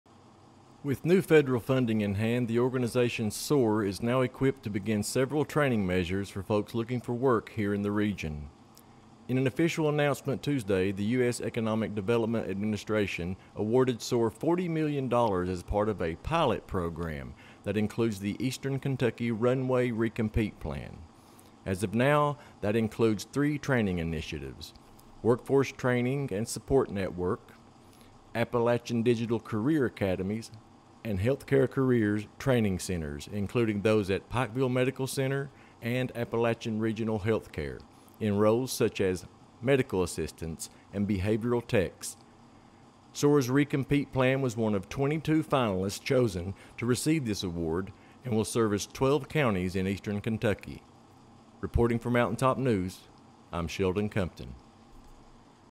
SOAR Receives Funding for new Training Measures
SOAR has received funding to begin new training measures for the region.